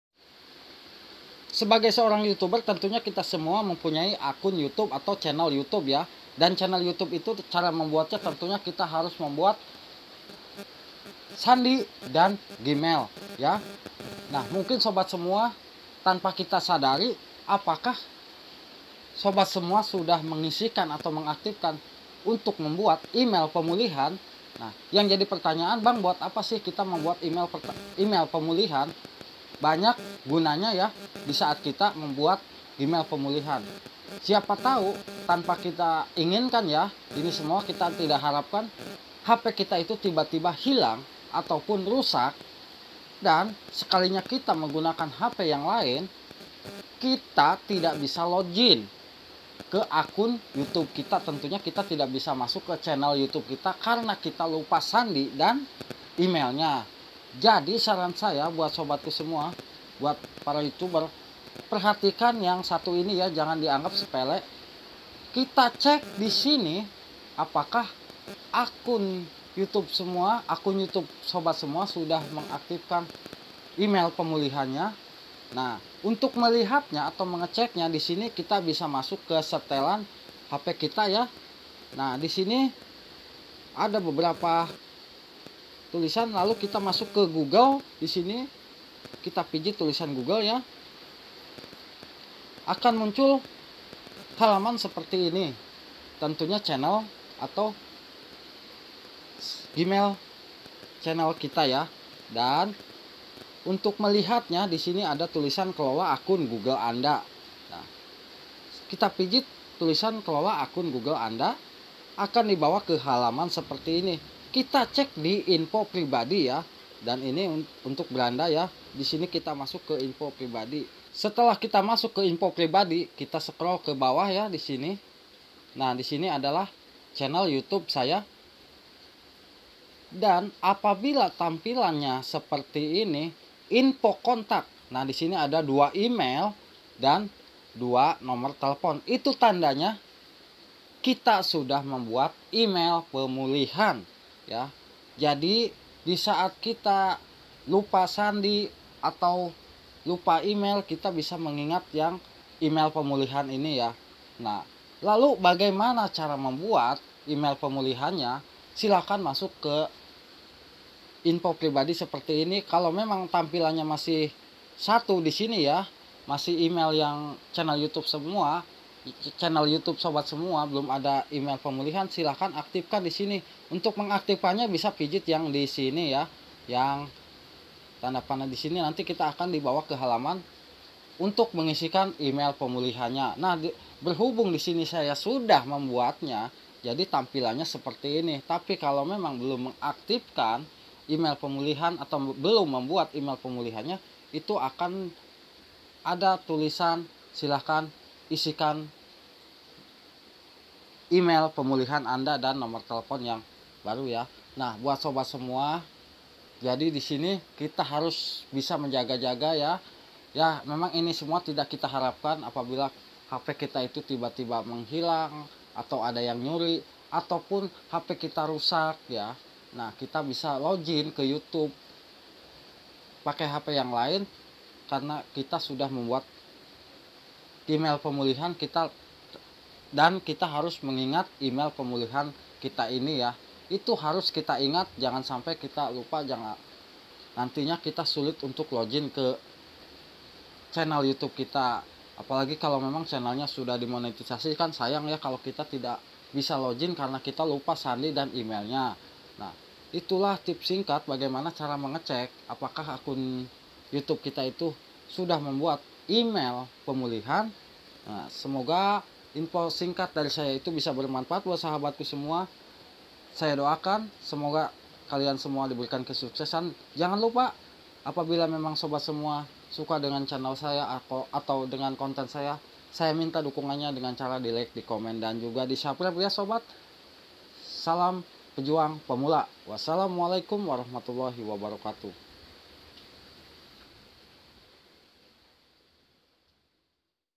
cara melihat dan membuat Email pemulihan akun google
untuk sobat semua yang memang mempunyai channel youtube apakah akun youtube anda sudah membuat email pemulihan nah di sini saya membuat vidio bagai mana cara melihat dan membuat email pemulihan coba cek sekarang akun google kalian semua